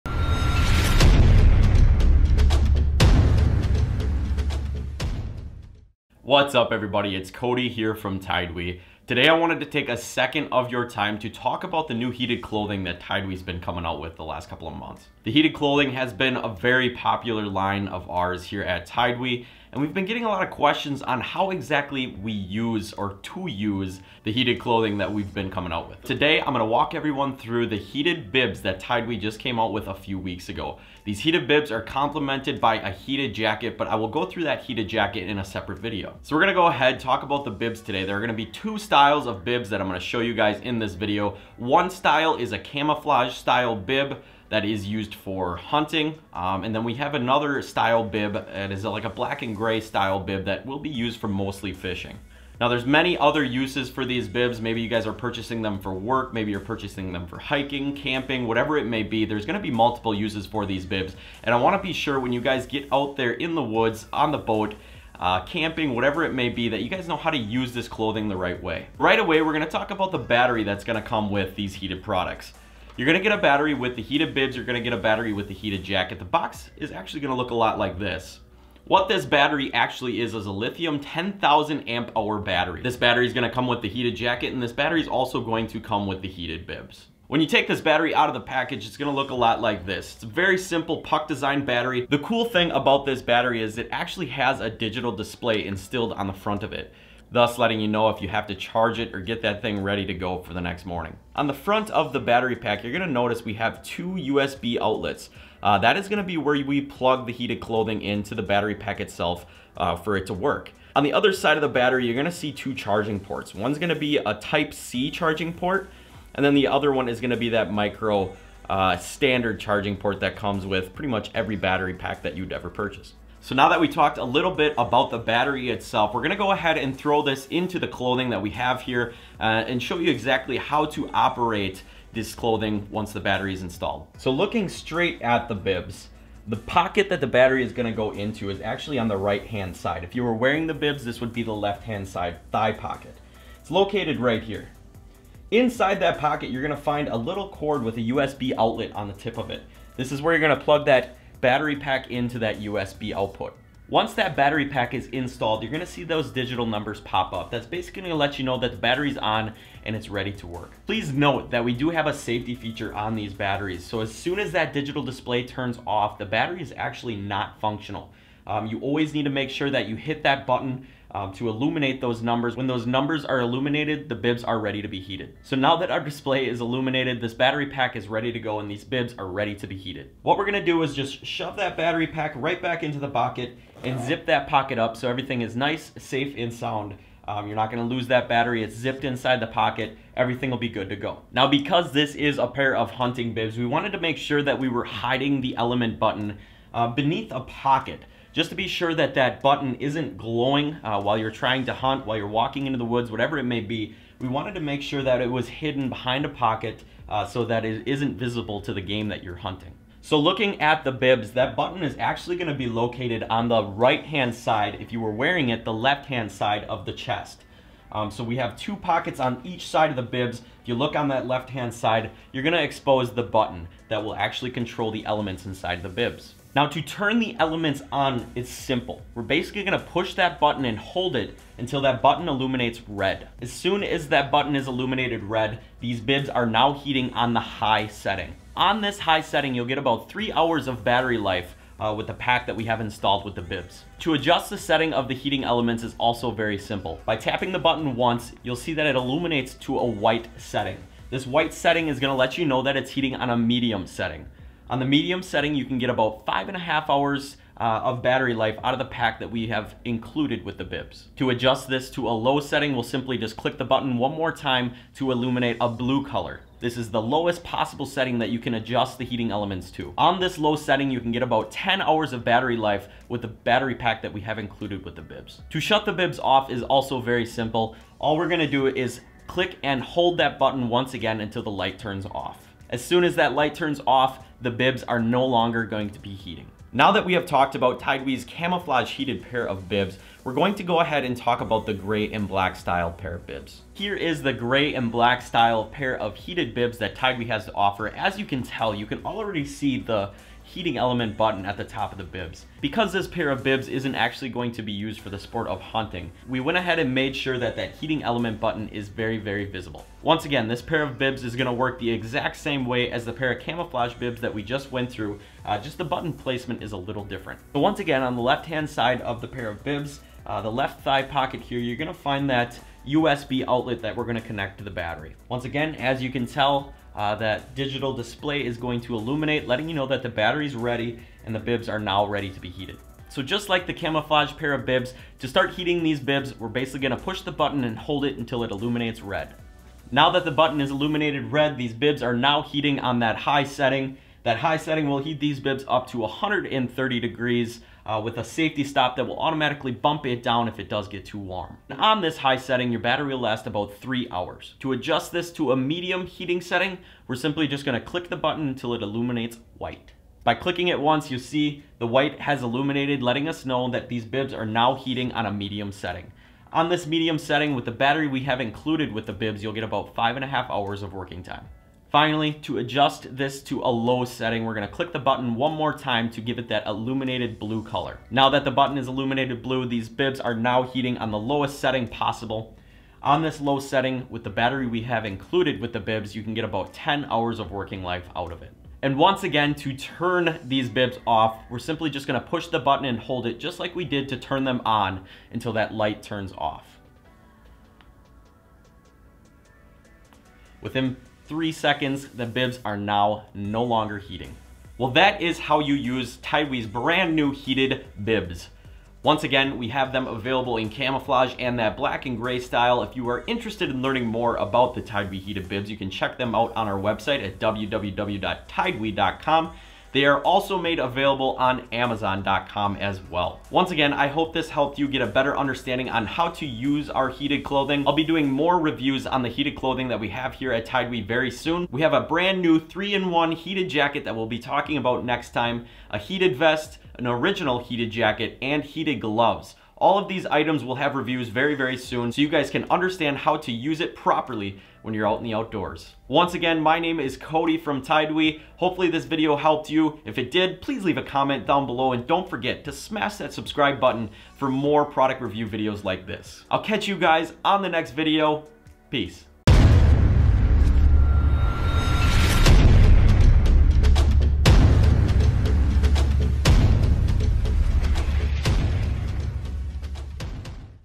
How To Use TideWe's Heated Bibs | Instructional Video
Hopefully this gives you a better understanding on how to use our heated bibs!

Amazon CA

TideWe is finally on Facebook and Instagram!!!